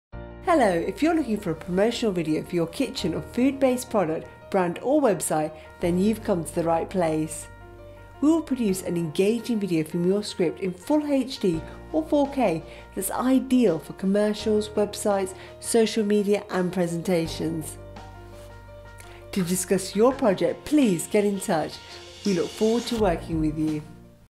Food Business Chef Promotional Tindy Spokesperson Video from Screen Promos
Hi, I'm Tindy, an actor and presenter from the UK. I work with a professional video production crew to provide high quality video promos

If you need a promotional video for your kitchen or food based product , business or brand, head over to Screen Promos and let us create an amazing video from just $25

This offer includes

    HQ Sound
    Shot on a Green Screen in professional studio

Custom options: ( no additional cost )

    Chef outfit
    Add your logo.
    Choose backgrounds from Kitchen, Green Screen or provide your own.